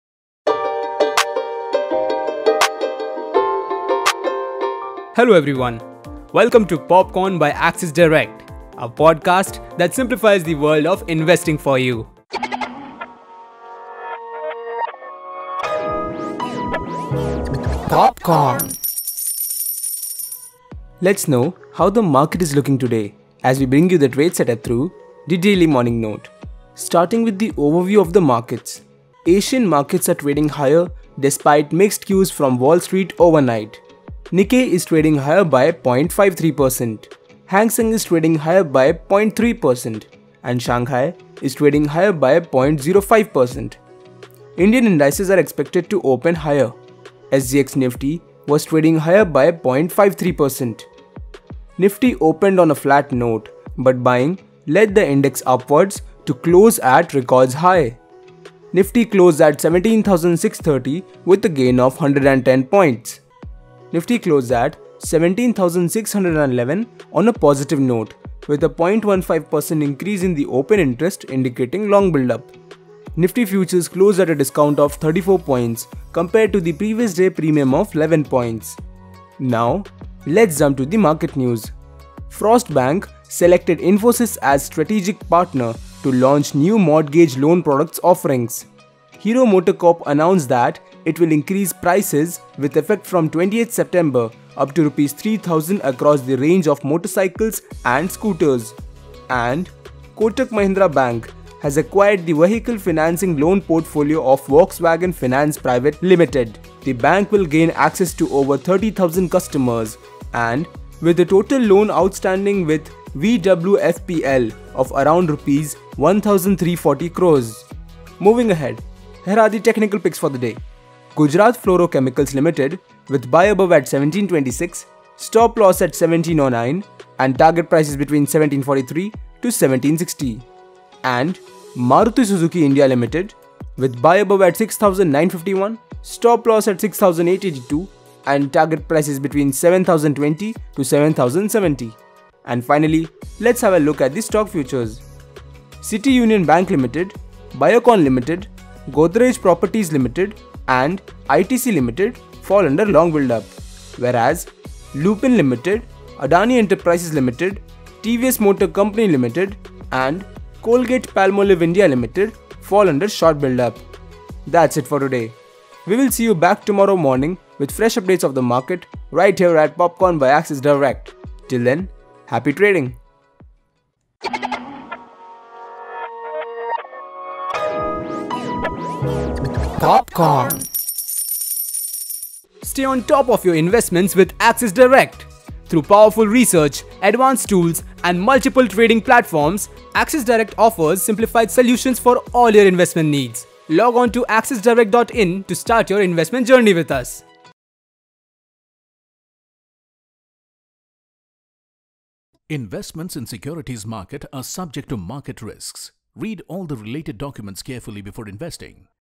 Axis Direct Presents Daily Morning Note - 17 September 2021
How is the market looking today? Get up to speed on the stock market with latest investment trends and news with our Daily Morning Note.